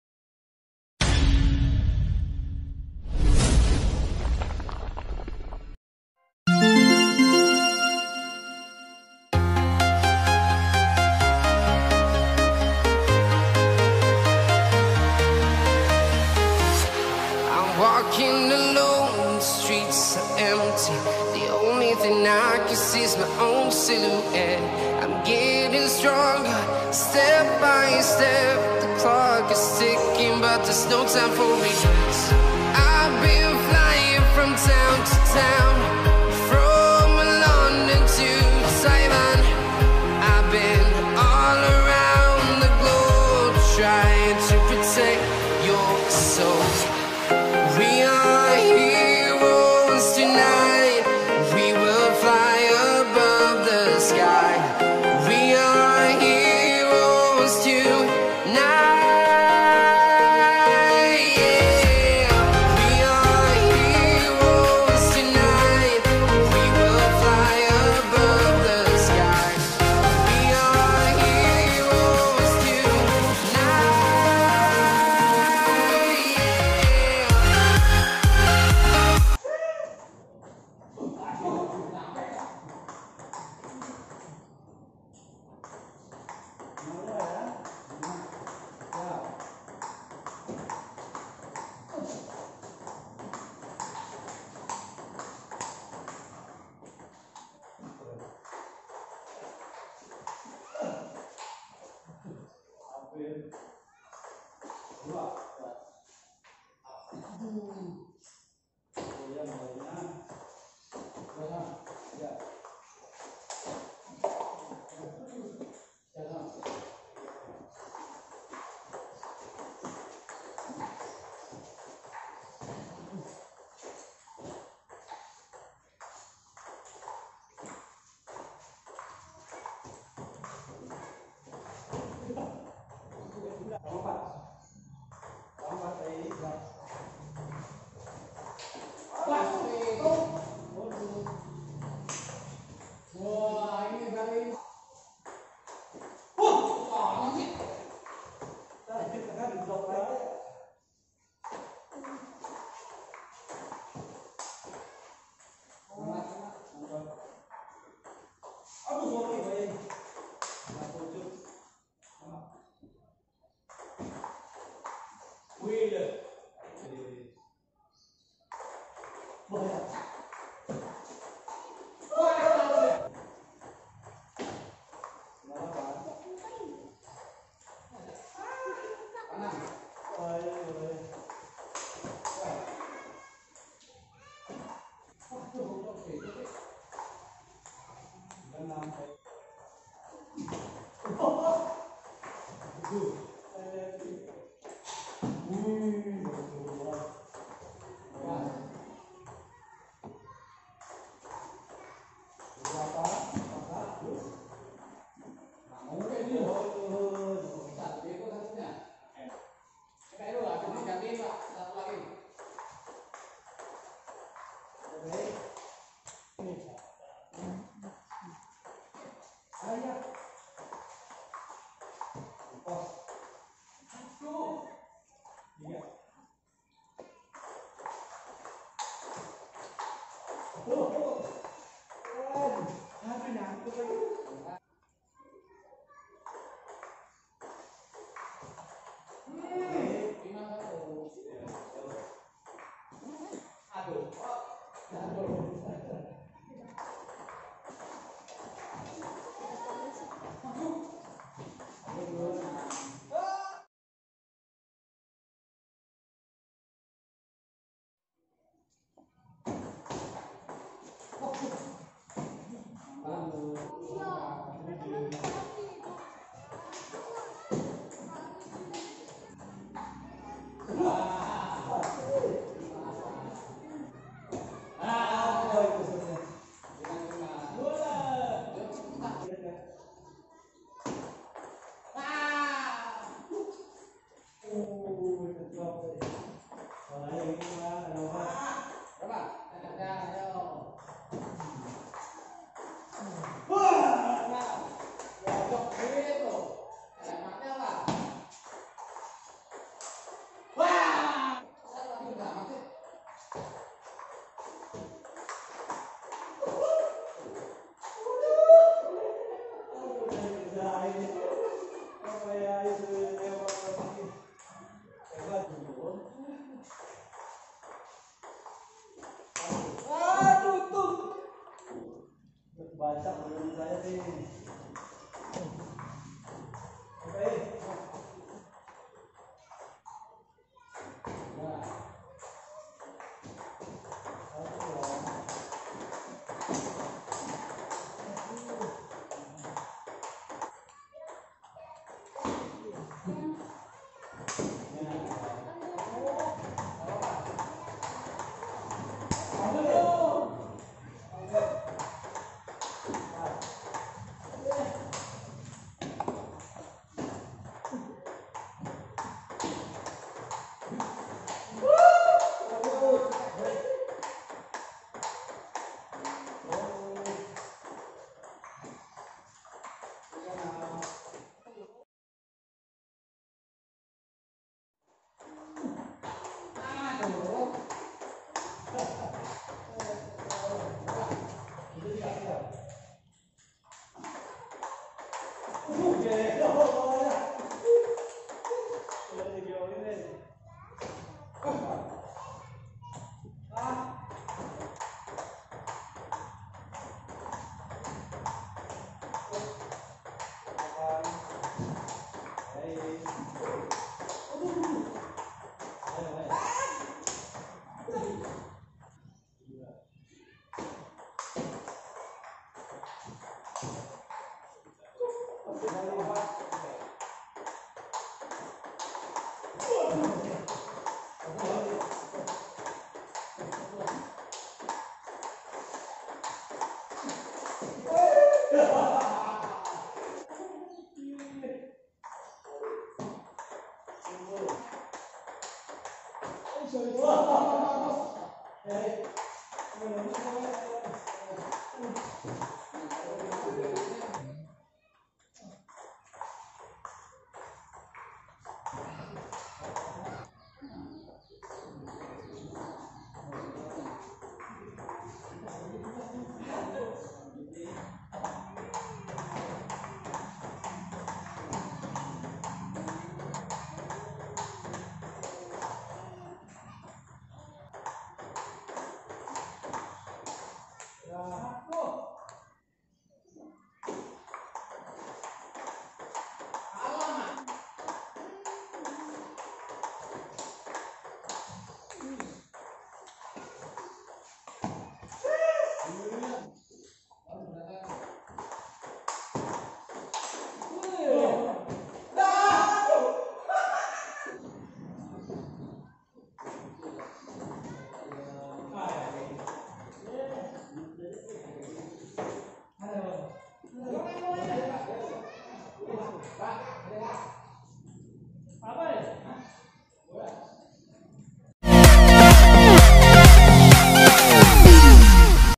Bintik Killer vs Bintik Pluto🏓Apoy Bintiker|YT.PTM Condet Raya vs Angger Pluto|YT.Tenis Meja Ciledug
Latihan Bersama Anggota KYP

Apoy Bintiker YouTube Channel PTM Condet Raya vs Angger Pluto YouTube Channel Tenis Meja Ciledug

di PTM Condet Raya

Bintik Killer vs Bintik Pluto

update elite turnament divisi 4++

semifinal pertama
elite turnament divisi 4++

Final Elite Turnament divisi 4++
Nael Niman (STONI) VS Irvan (RONDOPONG)